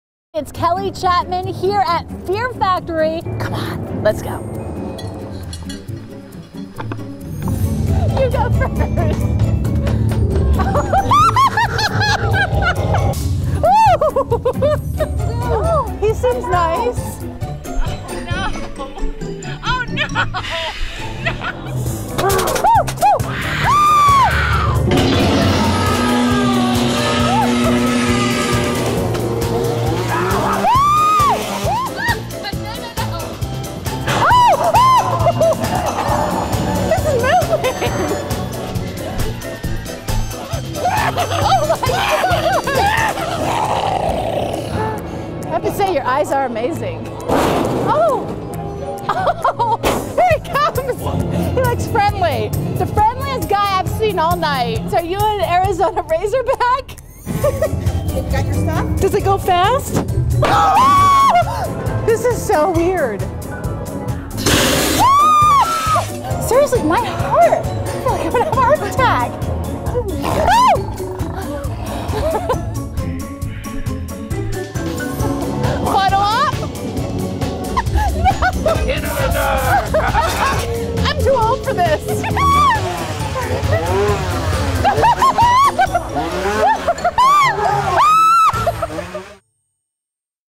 Fox 13's anchor Kelly Chapman visits Fear Factory
It's Kelly's first haunted house experience in 25 years!